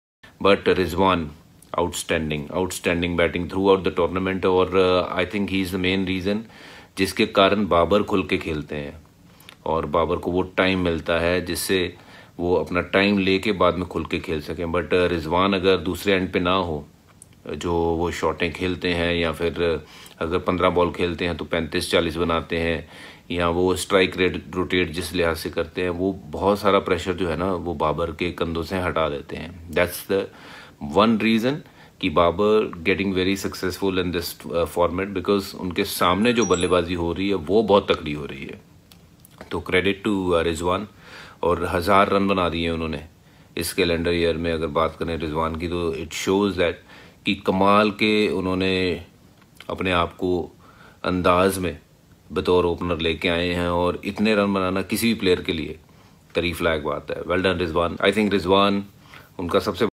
Muhammad Rizwan Ke Best Performance Par Bharti Khelari Harbhjan Singh Ki Dad
آخرکار پاکستانی بلے بازمحمدرضوان نے ہربھجن سنگھ کو ایسا کہنےپر مجبورکردیا